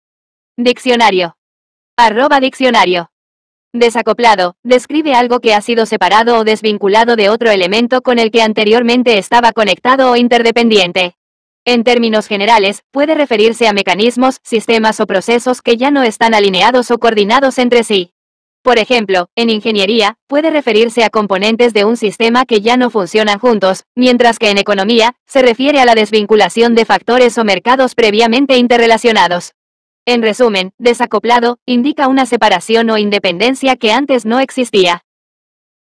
Desacoplado Significado En Español
"Desacoplado" describe algo que ha sido separado o desvinculado de otro elemento con el que anteriormente estaba conectado o interdependiente. En términos generales, puede referirse a mecanismos, sistemas o procesos que ya no están alineados o coordinados entre sí. Por ejemplo, en ingeniería, puede referirse a componentes de un sistema que ya no funcionan juntos, mientras que en economía, se refiere a la desvinculación de factores o mercados previamente interrelacionados. En resumen, "desacoplado" indica una separación o independencia que antes no existía.